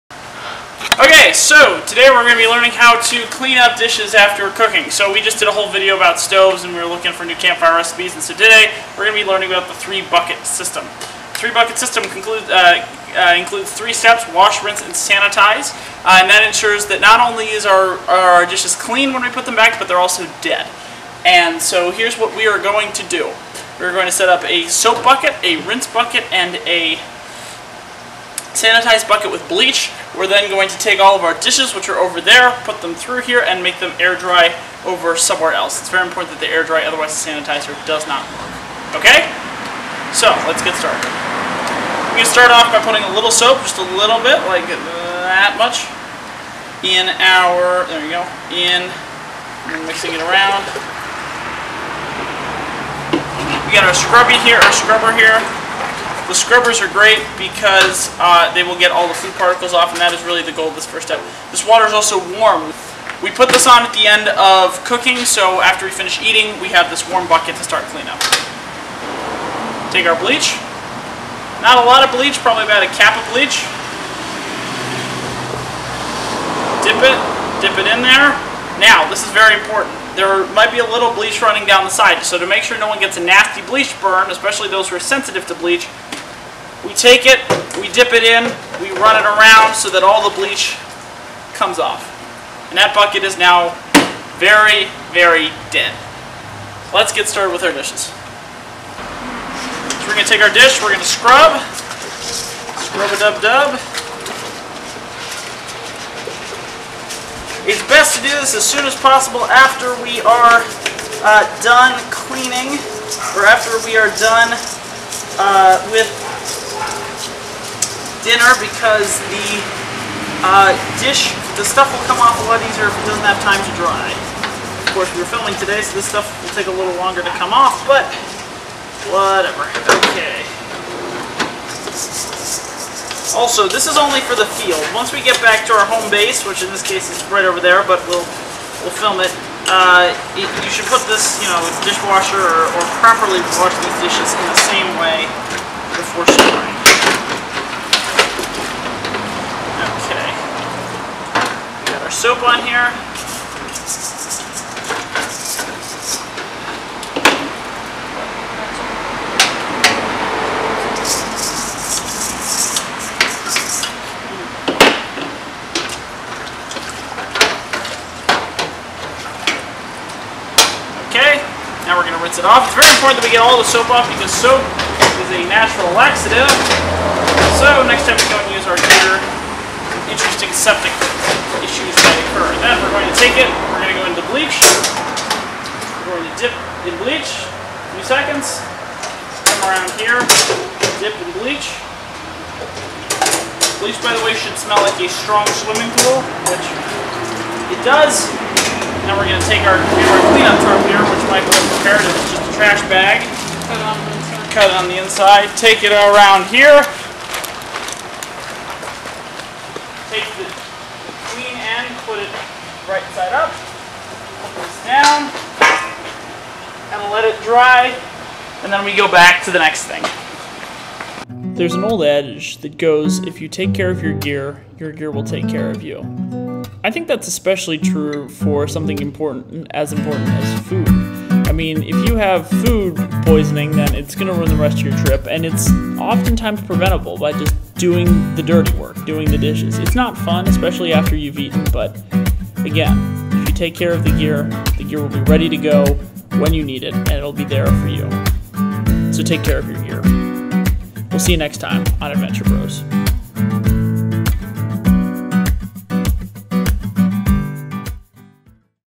Doing the Dishes | Adventure Bros
In this video, we're doing the dishes; John covers how to do dishes in the field and why you should take care of your gear.